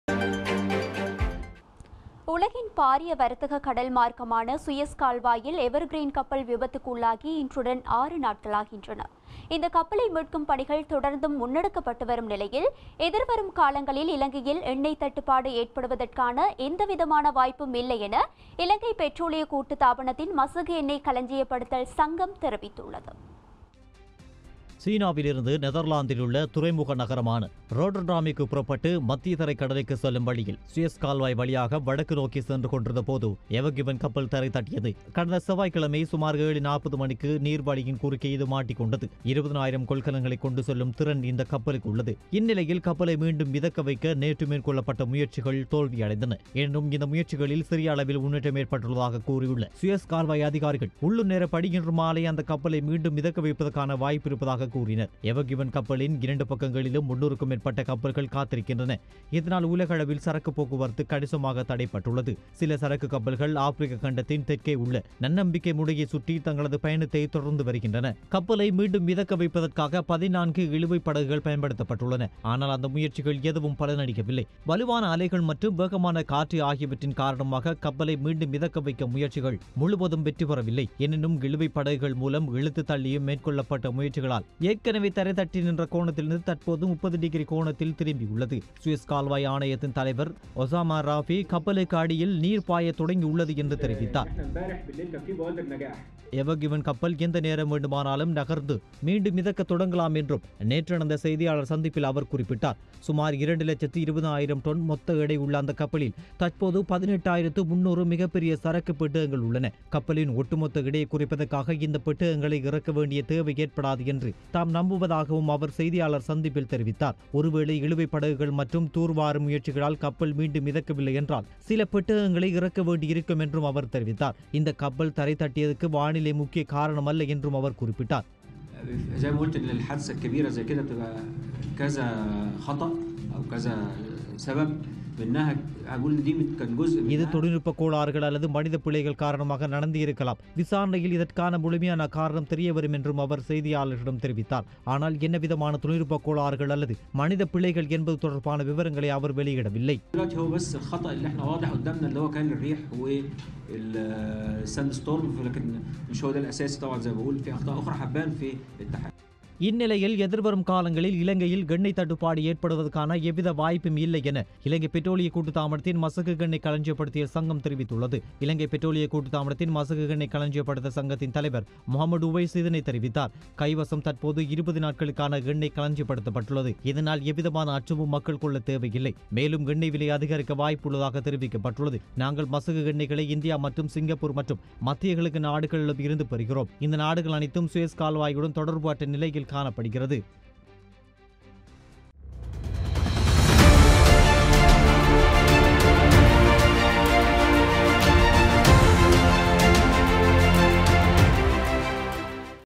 ‘எவர் க்ரீன்’ லேசாகத் திரும்பியது
கல்வி அமைச்சினால் வெளியிடப்பட்டுள்ள விசேட சுற்று நிரூபத்திற்கமைய சுகாதார பாதுகாப்பு விதிமுறைகளுடன் நாளை மேல் மாகாணத்தில் சகல வகுப்பு மாணவர்களுக்குமான கற்பித்தல் நடவடிக்கைகள் ஆரம்பமாகவுள்ளன.